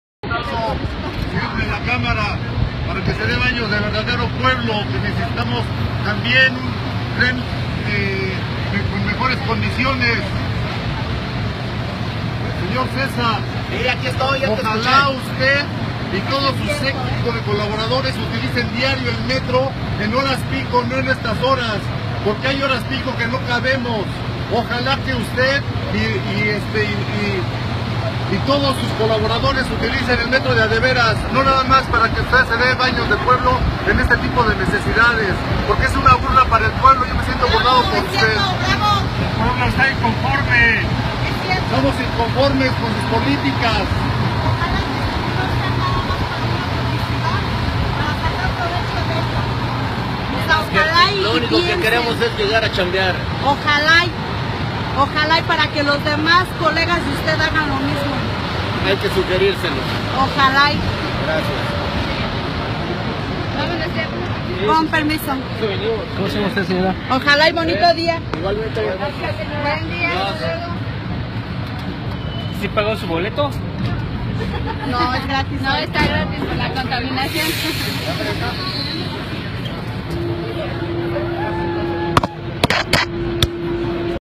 Reprenden en el Metro a diputados del PRI
Ante la restricción del uso del automóvil en la Ciudad de México debido a la contigencia ambiental, algunos diputados federales del PRI decidieron subirse al Sistema de Transporte Colectivo Metro... pero no fueron bien recibidos.